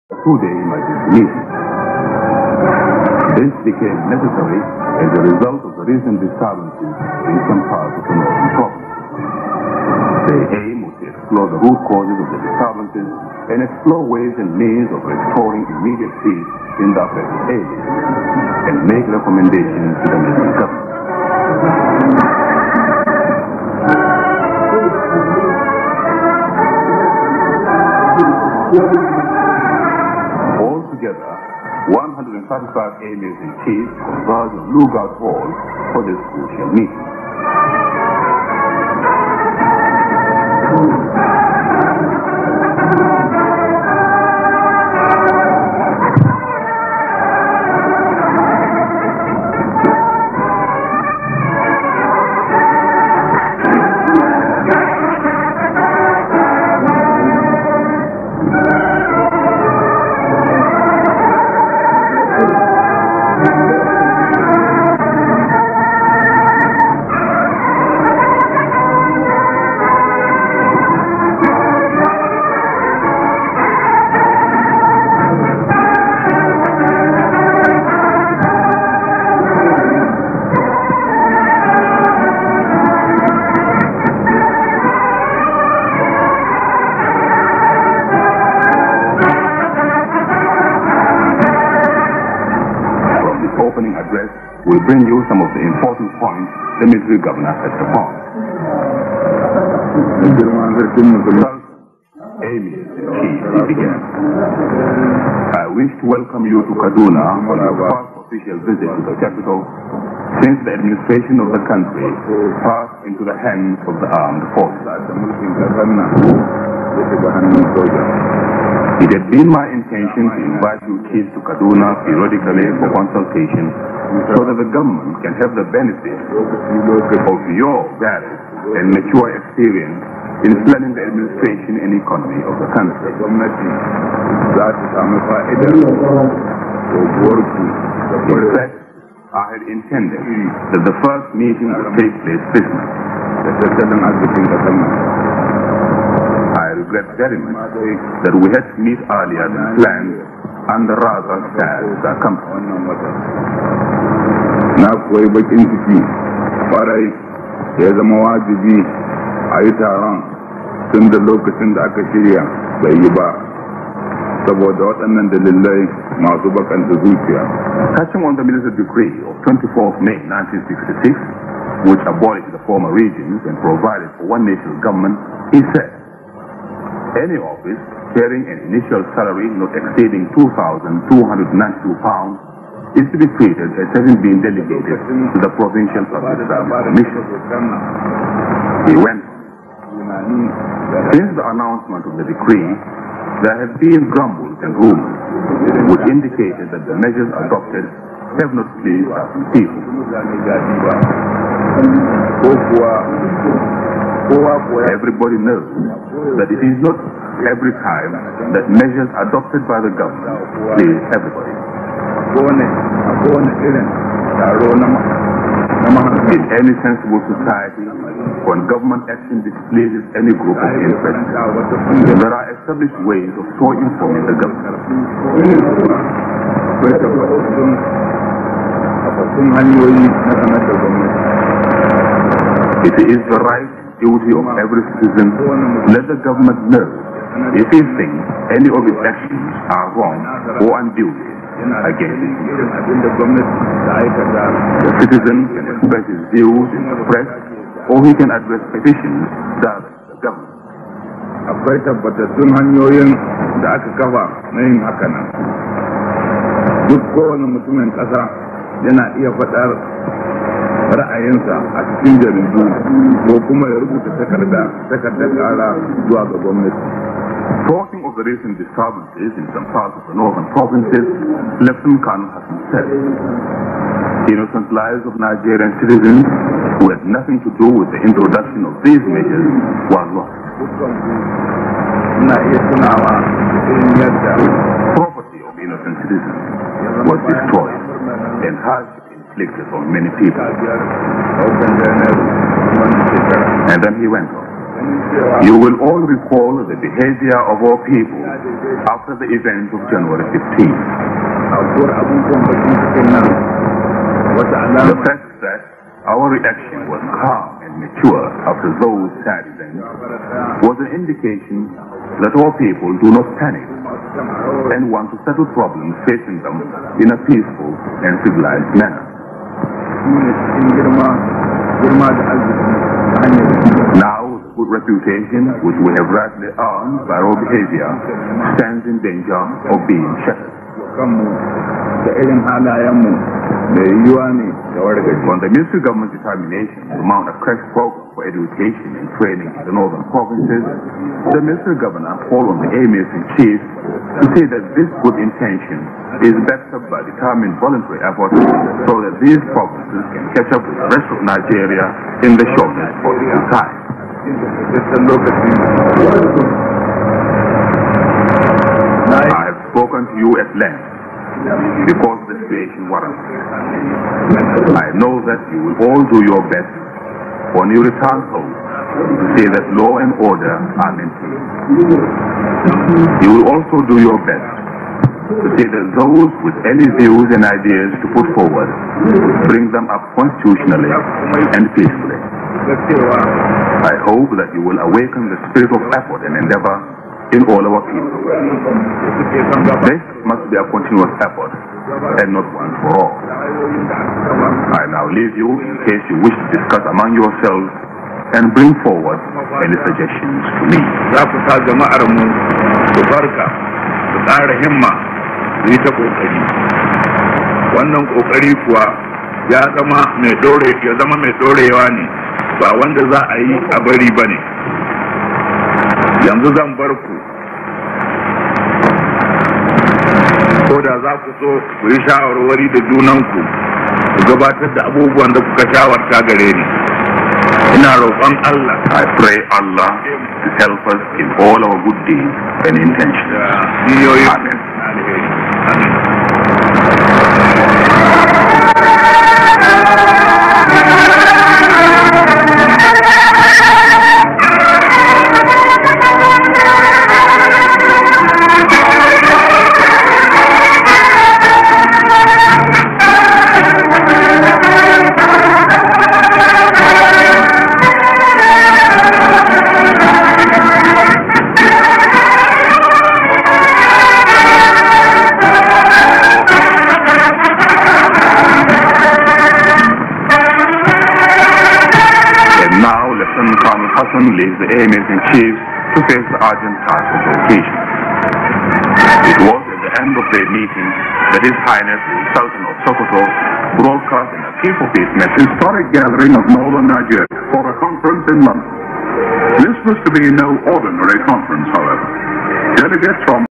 TARON FARKO BAYAN KASHE SU SARDAUNA 1966, GENERAL HASSAN KATSINA
First Meeting After 1966 Coup in Nigeria by General Hassan Katsina